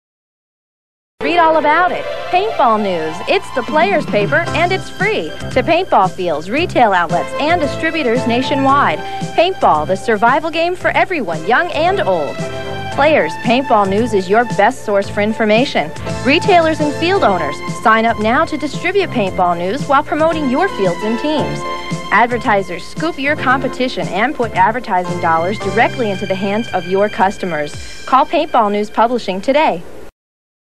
1992 Paintball News, old school newspaper ad!
The Paintball News was a newspaper dedicated to the sport of paintball. It featured articles on teams, fields and products for years. It was available at paintball stores and fields and before the internet took over it had the most up to date coverage of most events. I cool piece of paintball history. If I remember it was put out by Rene Bouche (please correct me if I'm wrong).